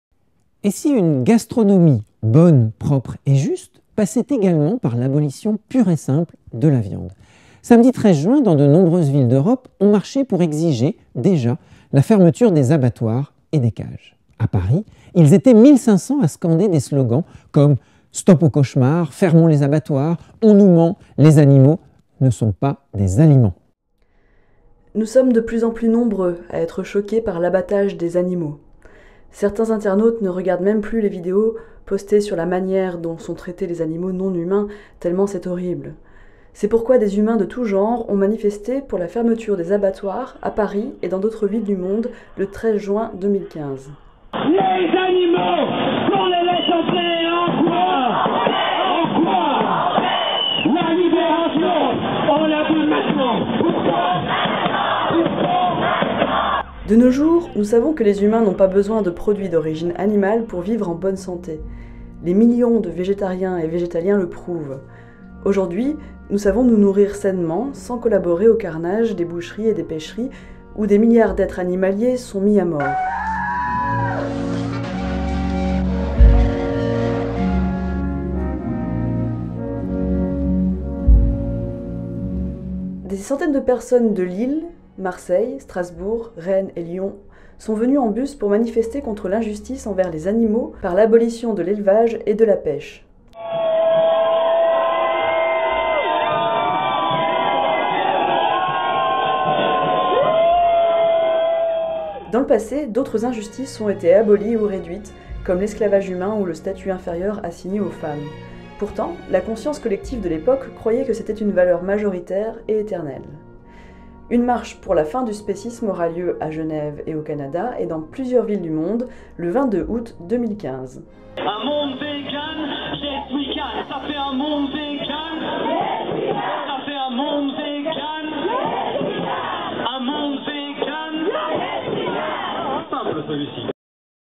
Fermons les abattoirs - Reportage Alter JT 18/06/15 - 52
"On nous ment, les animaux ne sont pas des aliments"
ou "Stop au cauchemar, fermons les abattoirs"
des slogans clairs et simples ont rempli plusieurs villes dans le monde samedi 13 juin 2015 et notamment à Paris.
La manifestation a proposé de fermer les cages et d'ouvrir les esprits.

un reportage fait par Joyce